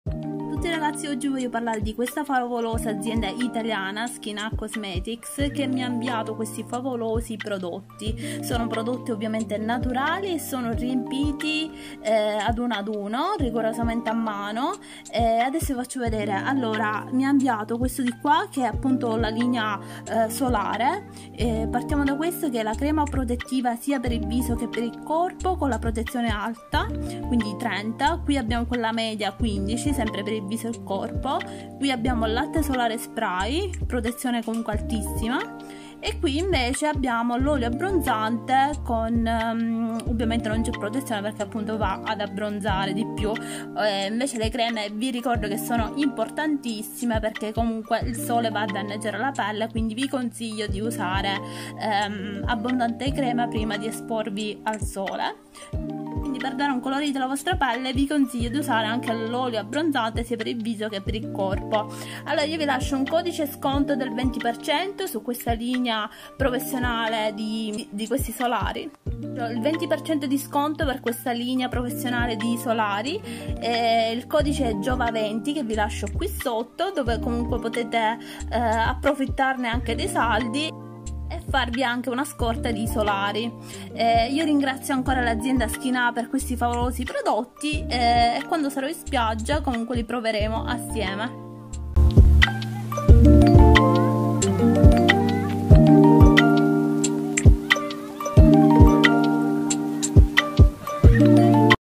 La nuova Linea solari professionali Skinà cosmetic - Unboxing
Da anni i laboratori di SKINA’ COSMETICS mettono al primo posto 'LA NATURA' il miglior alleato per la nostra pelle...

Vi presento la Nuova Linea Solari professionale con Ingredienti Naturali

●Sun Protect – Crema protezione Alta SPF 30

●Sun Protect – Latte solare protezione SPF 50

●Sun Protect – Crema protezione Media SPF 15

●Sun Oil – Olio abbronzante solare spray

'GIOVA20'